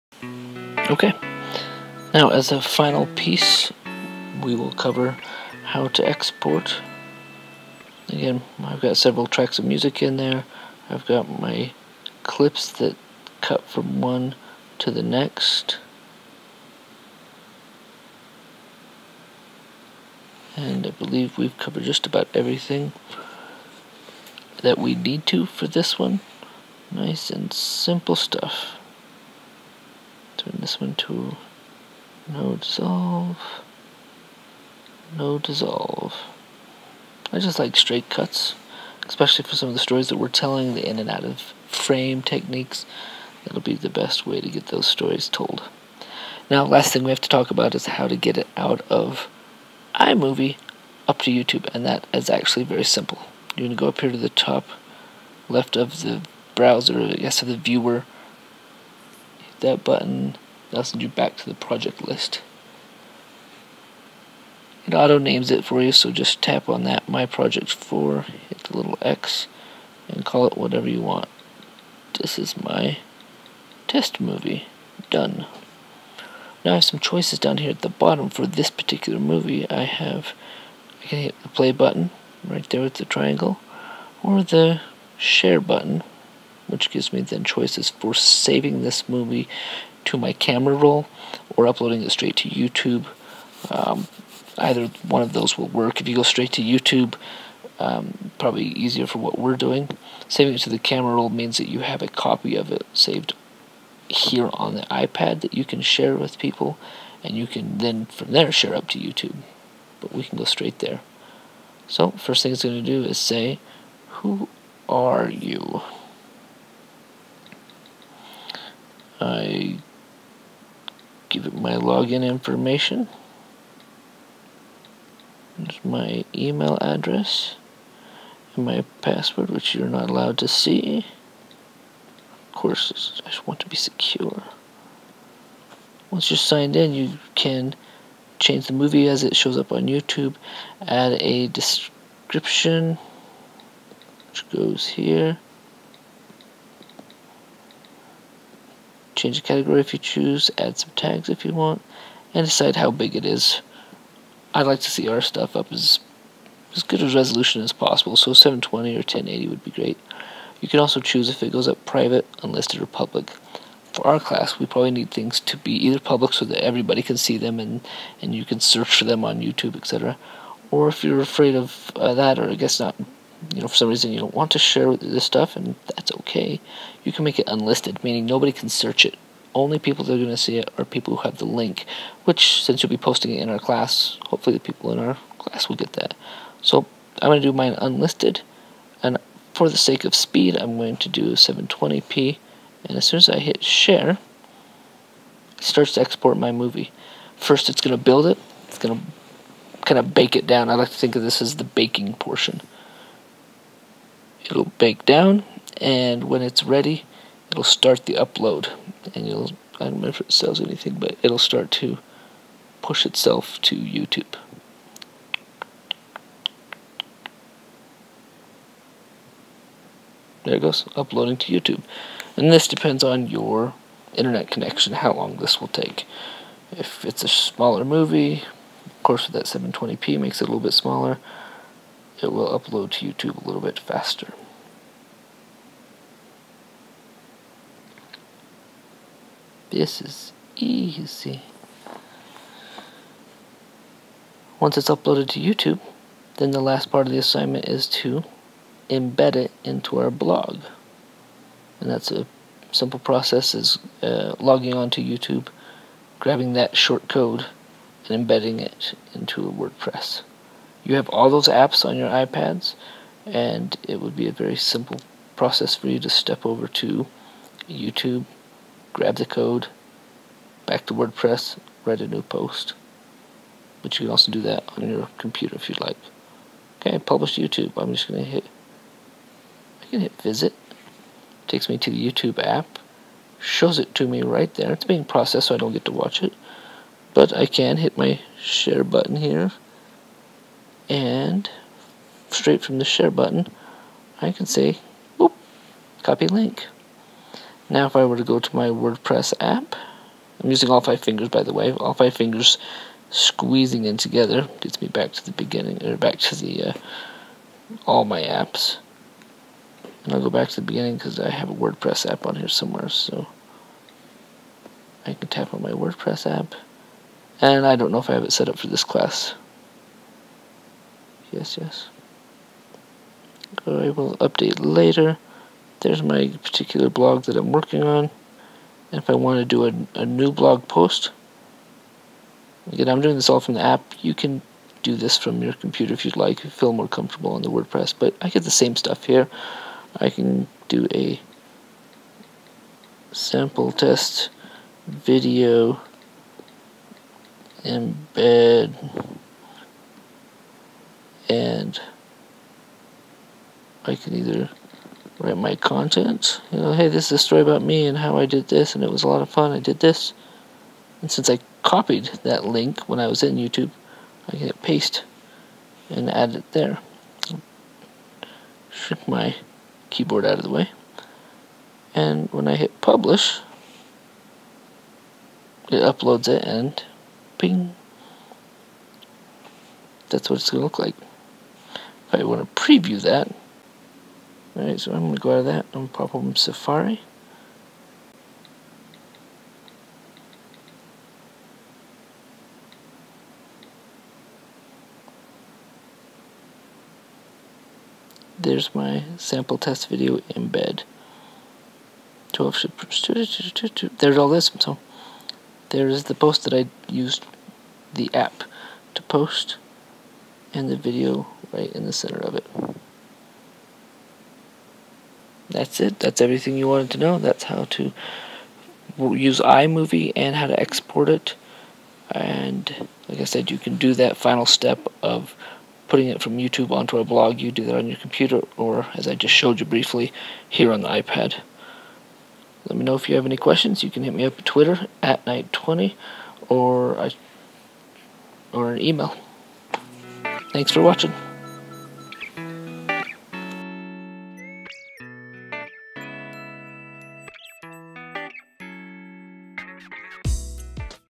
iMovie exporting to YouTube- embed into wordpress on iPad
This is a walkthrough for exporting your videos from iMovie to YouTube and then embedding into wordpress for MPC 6300